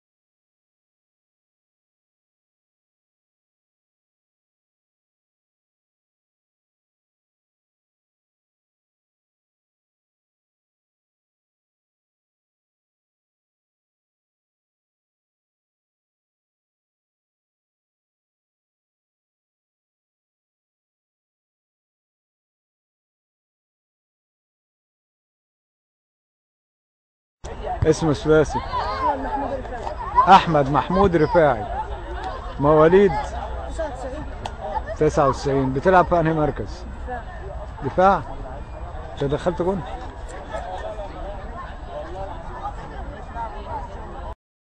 اللاعب احمد محمود رفاعي مواليد1999
اضغط
لرؤية الفيديو الخاص بالاعب احمد محمود رفاعي مواليد1999 الذي تم اختياره من الجهاز الفني للعب مع الفريق اكاديمية نادي 6 اكتوبر لعام 2013 / 2014 للعب مع البراعم لمنطقة  الجيزة  للاتحاد المصري  لكرة القدم مع تحيات سمير لبيب